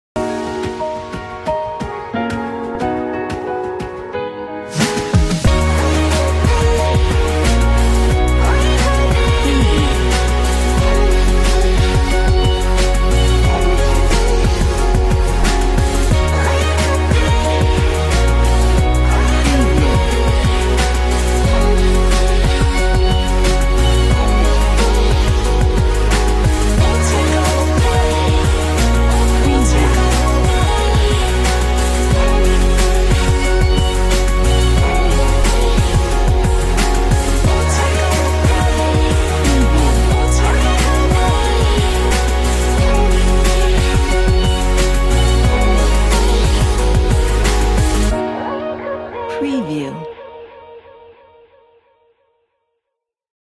Cinematic Reel Premiere Pro Templates
Cinematic Reel is a Premiere Pro template with 22 media and 12 editable text layers. It is stylishly designed and has creatively simple text animations. Ideal as a demo of your work, intro or promo, slideshow, TV show, trailers, presentations, teasers, products, advertisements, broadcasts. Create original, inspirational, and cinematic slideshows in no time using this template.